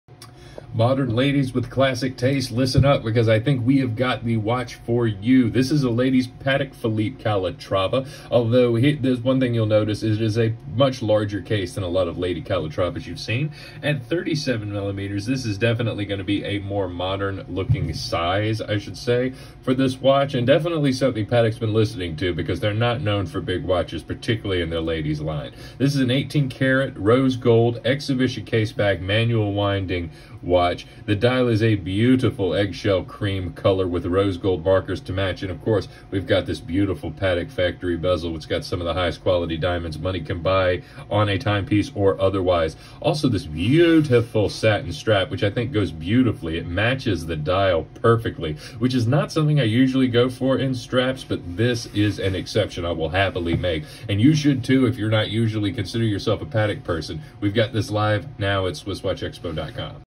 Patek Philippe Calatrava Rose Gold Beige Dial Diamond Ladies Watch 4897R Review | SwissWatchExpo
The Patek Philippe Calatrava is the quintessential dress watch. This ladies version comes in an 18k rose gold case and a bezel with Patek factory diamonds. It also features a beige lacquered guilloche dial with gold baton hour markers and dauphine hands.

FULL DETAILS AND PRICING:
Patek Philippe Calatrava Rose Gold Beige Dial Diamond Ladies Watch 4897R

18k rose gold case 33.0 mm in diameter. Exhibition transperrent sapphire crystal case back. 18k rose gold Patek factory diamond bezel. Beige lacquered guilloche dial with gold baton hour markers and dauphine hands.